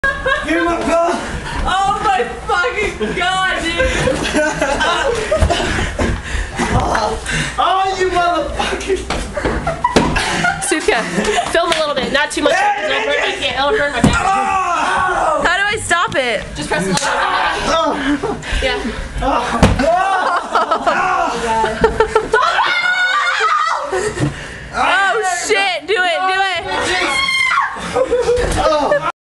Hotel Trashing!
Surf Expo '06 gets dirty in the hotel room. Too much Jack 'n coke? Probably so..